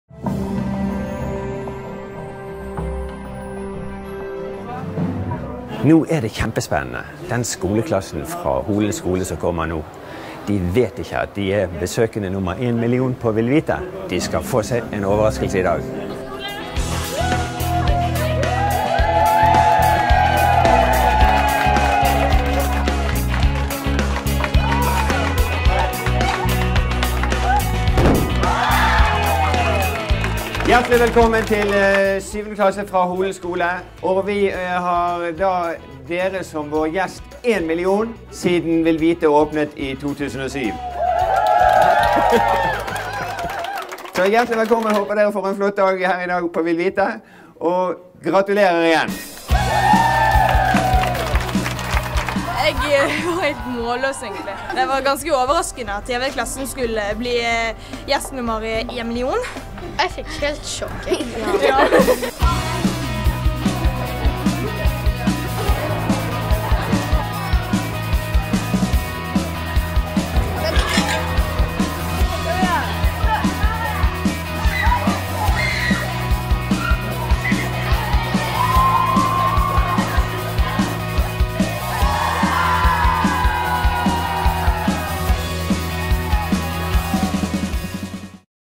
VilVite gjest nr. 1 million
Gjest nr. 1 million ankommer VilVite. For en overraskelse det ble for 7. trinn ved Holen skole i Bergen.